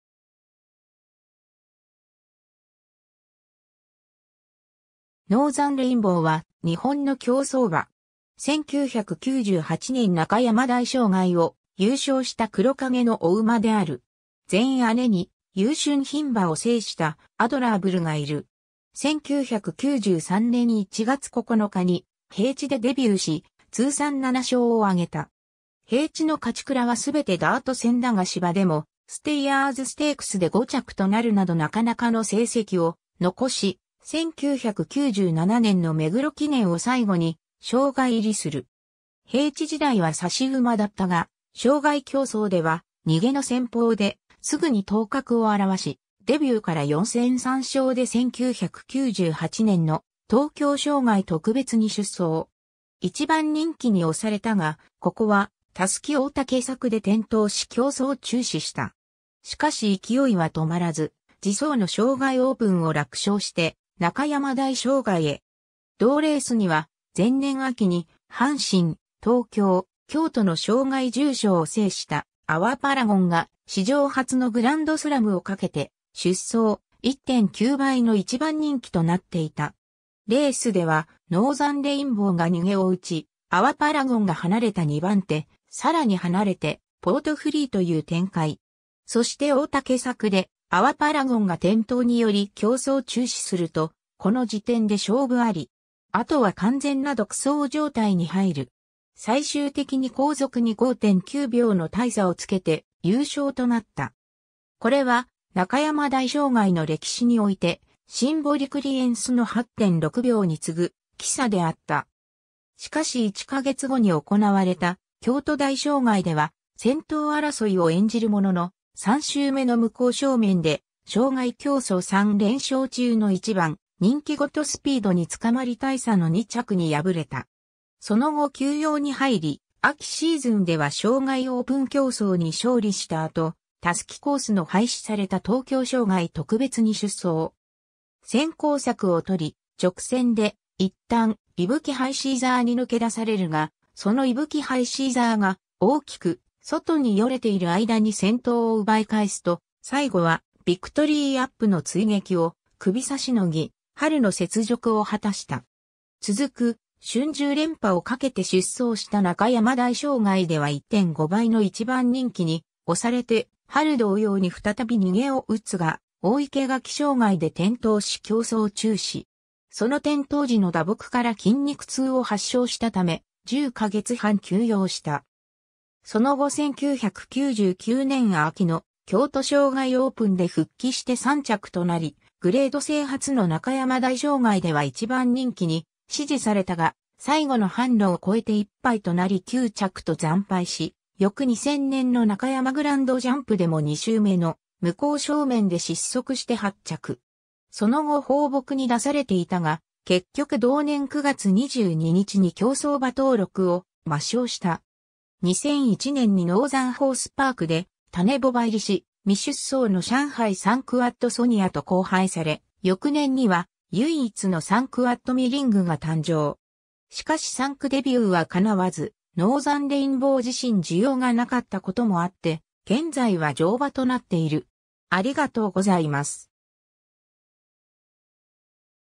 ノーザンレインボー
ノーザンレインボーは日本の競走馬。
1998年中山大障害（春）を優勝した黒鹿毛の牡馬である。
全姉に優駿牝馬を制したアドラーブルがいる。
 1993年1月9日に平地でデビューし通算7勝を挙げた。
平地の勝ち鞍は全てダート戦だが芝でもステイヤーズステークスで5着となるなどなかなかの成績を残し、1997年の目黒記念を最後に障害入りする。
 平地時代は差し馬だったが障害競走では逃げの戦法ですぐに頭角を現し、デビューから4戦3勝で1998年の東京障害特別（春）に出走。
1番人気に推されたが、ここは襷大竹柵で転倒し競走中止した。
 しかし勢いは止まらず次走の障害オープンを楽勝して中山大障害（春）へ。
同レースには前年秋に阪神、東京、京都の障害重賞を制したアワパラゴンが史上初のグランドスラムを賭けて出走、1.9倍の1番人気となっていた。
レースではノーザンレインボーが逃げを打ち、アワパラゴンが離れた2番手、更に離れてポートフリーという展開。
そして大竹柵でアワパラゴンが転倒により競走中止すると、この時点で勝負あり。
後は完全な独走状態に入る。
最終的に後続に5.9秒の大差をつけて優勝となった。
これは中山大障害の歴史においてシンボリクリエンスの8.6秒に次ぐ着差であった。
 しかし1か月後に行われた京都大障害（春）では先頭争いを演じるものの、3周目の向こう正面で障害競走3連勝中の1番人気ゴッドスピードに捕まり大差の2着に敗れた。
その後休養に入り、秋シーズンでは障害オープン競走に勝利した後、襷コースの廃止された東京障害特別（秋）に出走。
先行策をとり、直線で一旦イブキハイシーザーに抜け出されるが、そのイブキハイシーザーが大きく外にヨレている間に先頭を奪い返すと、最後はビクトリーアップの追撃をクビ差しのぎ、春の雪辱を果たした。
 続く春秋連覇を懸けて出走した中山大障害（秋）では1.5倍の1番人気に推されて春同様に再び逃げを打つが、大生垣障害で転倒し競走中止。
その転倒時の打撲から筋肉痛を発症したため、10か月半休養した。
その後1999年秋の京都障害オープンで復帰して3着となり、グレード制初の中山大障害では1番人気に支持されたが最後の坂路を越えて一杯となり9着と惨敗し、翌2000年の中山グランドジャンプでも2周目の向こう正面で失速して8着。
その後放牧に出されていたが、結局同年9月22日に競走馬登録を抹消した。
 2001年にノーザンホースパークで種牡馬入りし、未出走のシャンハイ産駒ワットソニアと交配され、翌年には唯一の産駒ワットミリングが誕生。
しかし産駒デビューは叶わず、ノーザンレインボー自身需要が無かった事もあって、現在は乗馬となっている。